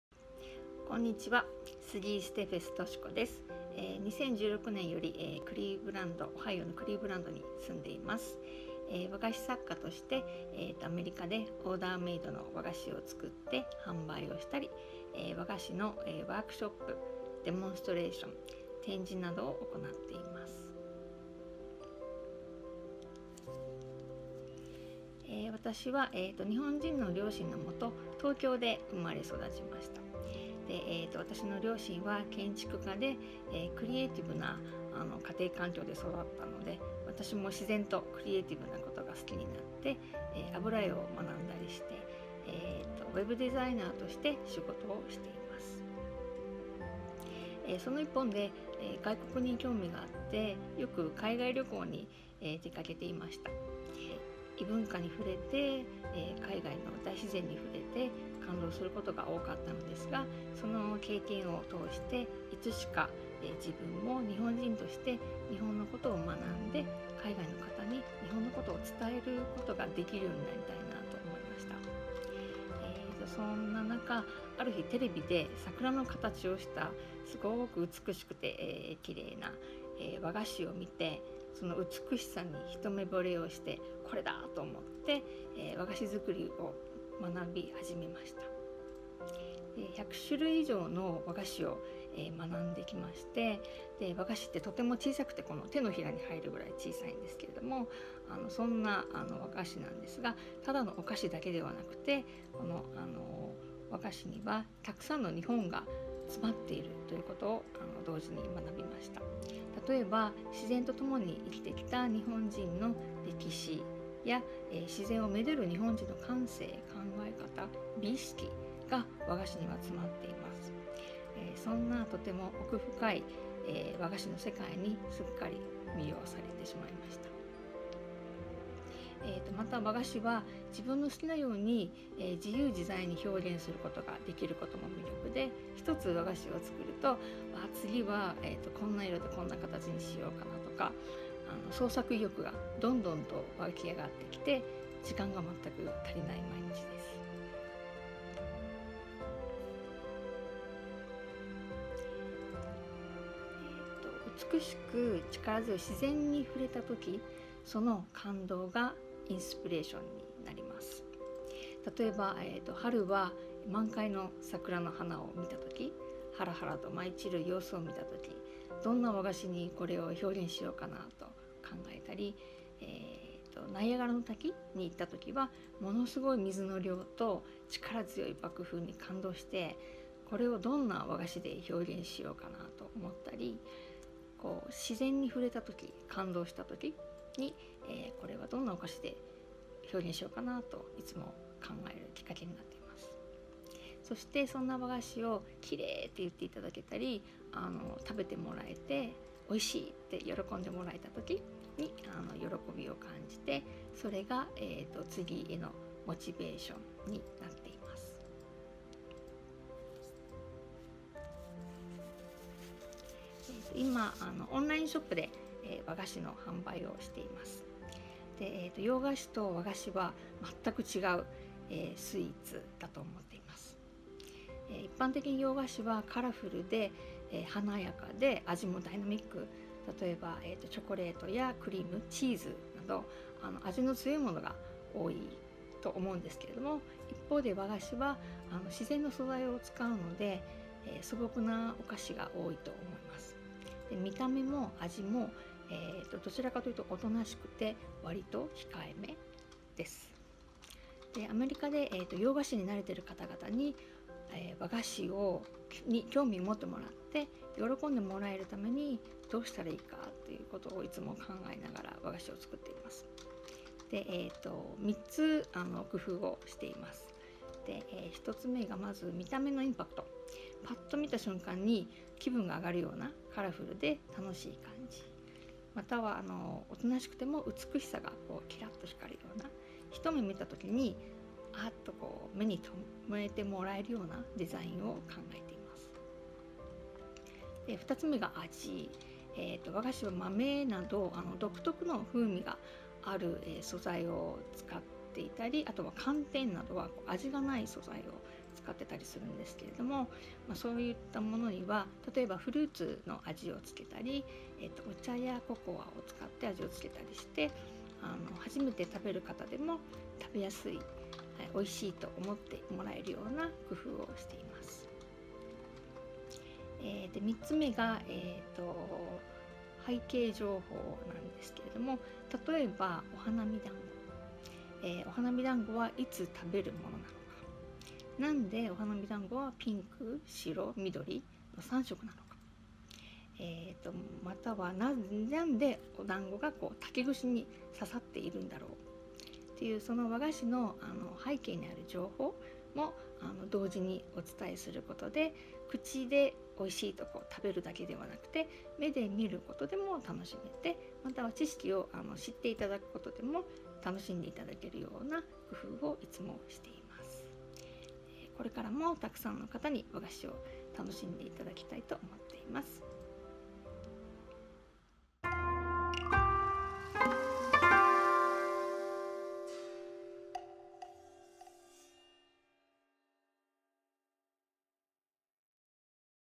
Women entrepreneurs in Ohio Series - 'Wagashi' Designer Ms.Toshiko Sugii Steffes
In one of the rarest videos, Toshiko San tells about her journey with Wagashi making & designing and takes us through the a quick history and deep meaning hidden in these small beautiful pieces of art 'Wagashi'!!